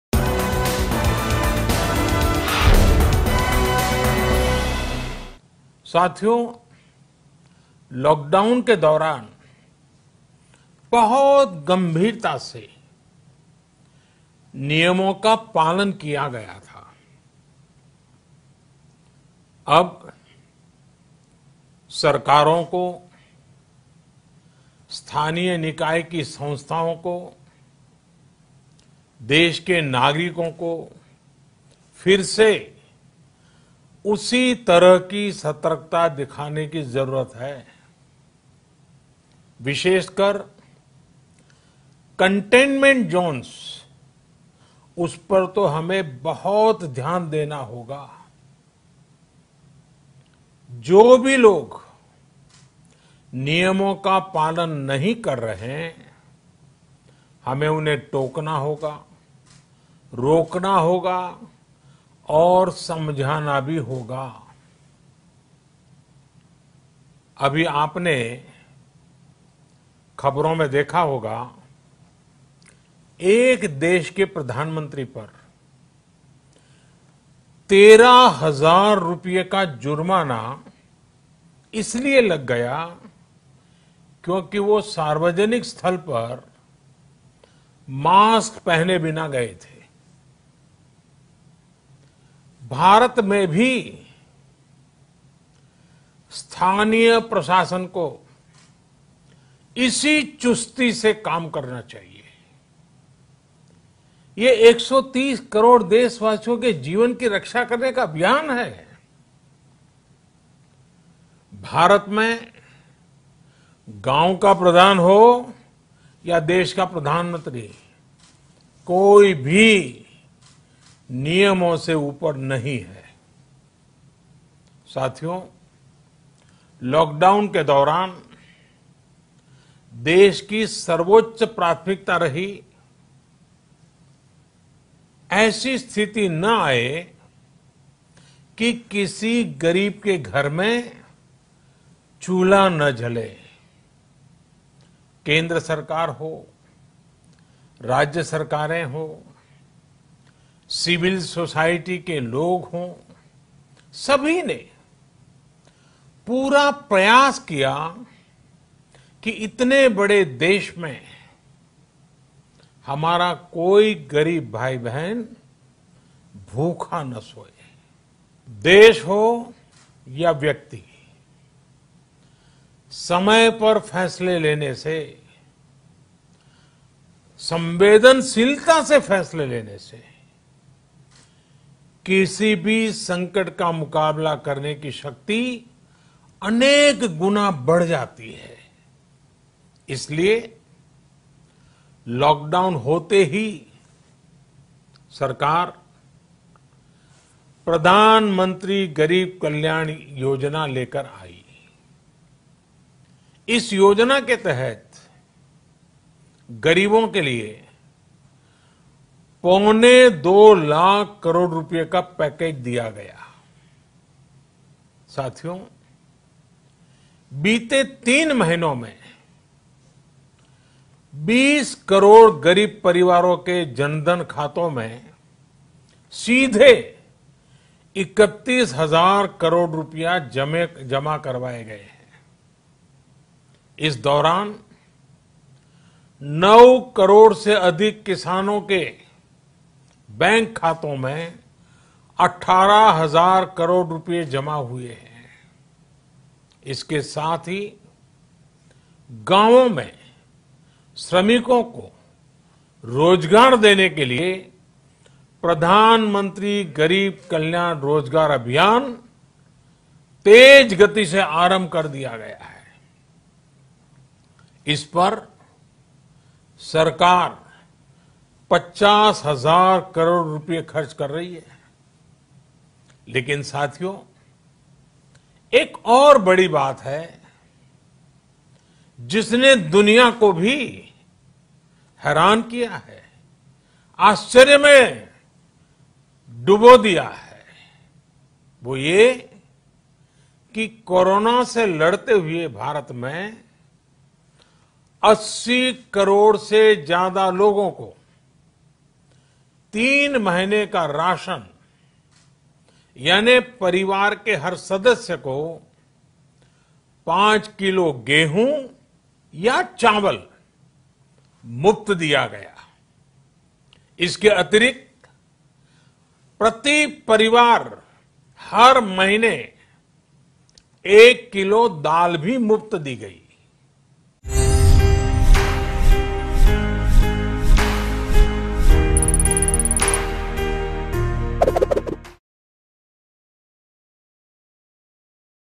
PM Modi Says Other Country PM Fined Over Not Wearing Mask In Public Places | Covid19 | Mango News
PM Modi Says Other Country PM Fined Over Not Wearing Mask In Public Places.